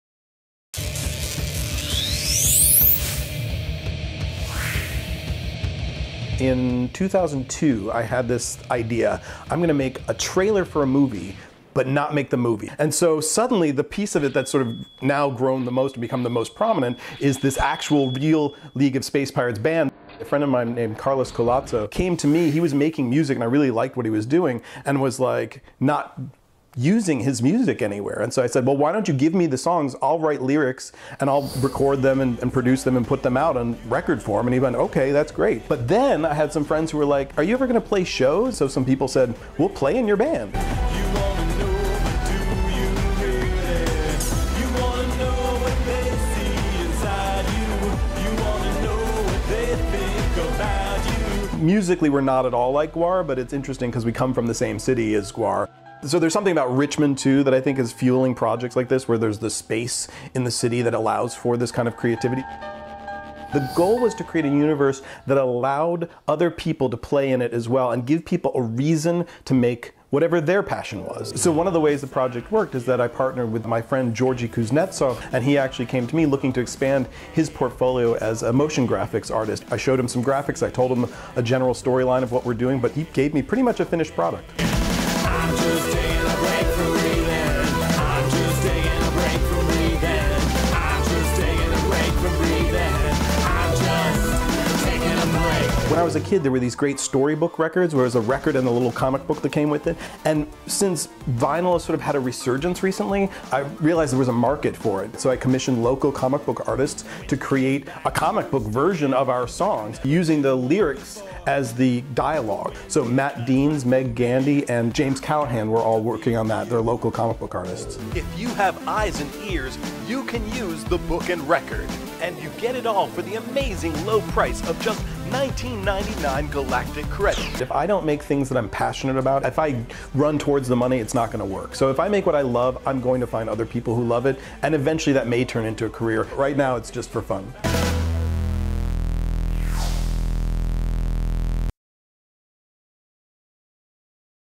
Noah Scalin
Noah Scalin is best known for his Skull-A-Day art project, but he's expanded into music: live, video & vinyl.